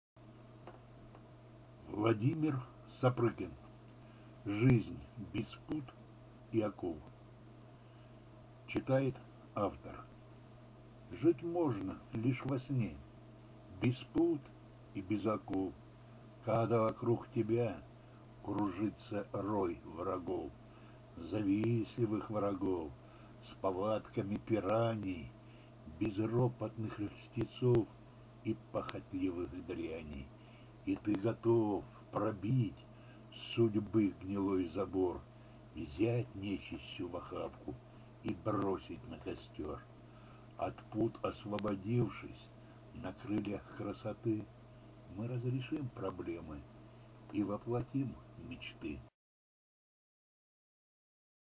Жизнь без пут и оков
стихотворение Владимира Сапрыкина, читает автор